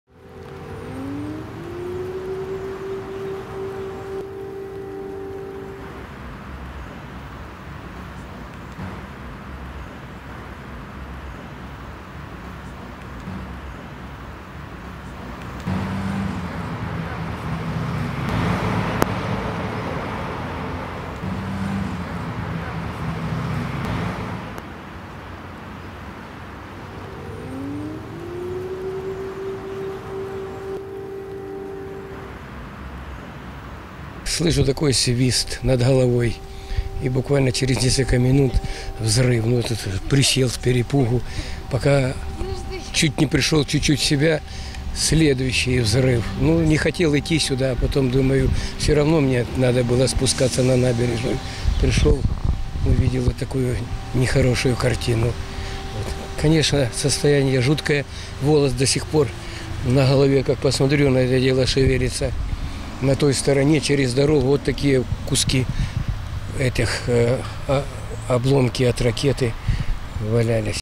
💥 АТАКА по Запорожью: оккупанты РФ попали в местный отель
В результате вчерашней российской ракетной атаки по Запорожью погиб один человек, 19 получили ранения. Среди пострадавших — четверо детей. Оккупанты нанесли удар по отелю "Reikartz Запорожье".

Спасатели ликвидировали возгорание автомобиля и эвакуировали посетителей поврежденного здания.

Кадры последствий атаки – в видео FREEДОМ.